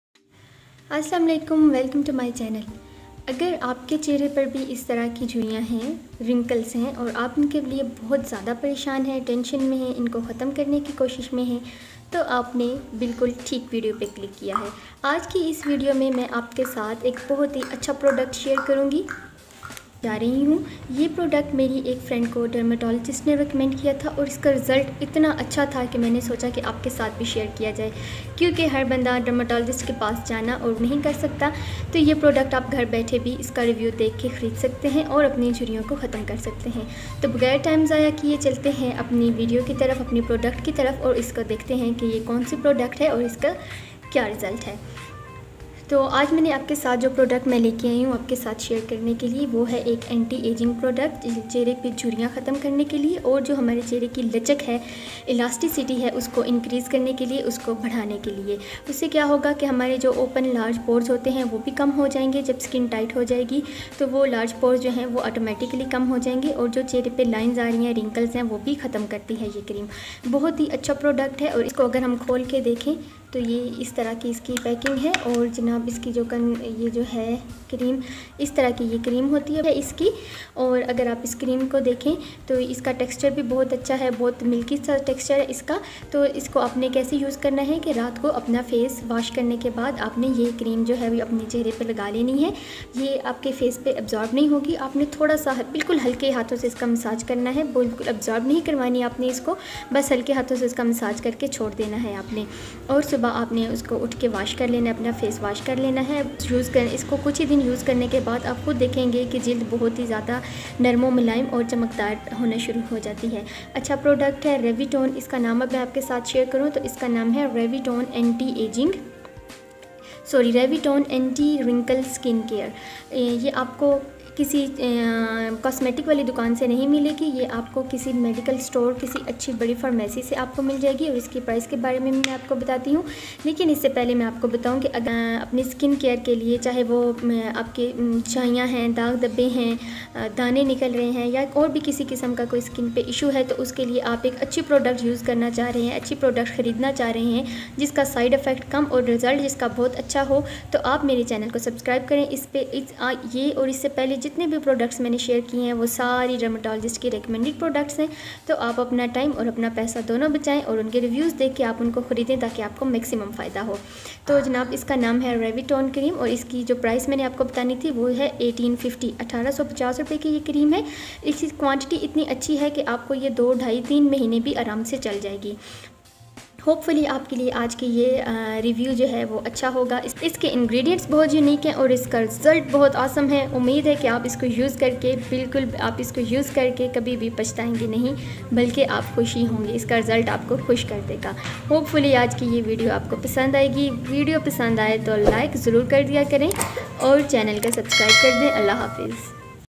Best anti aging cream in pakistan2022|How to remove wrinkles
Best anti aging cream in pakistan|
How to remove wrinkles
in this video i share a cream for anti aging and aniti wrinkles .this will help you guys to choose best product for anti aging and wrinkles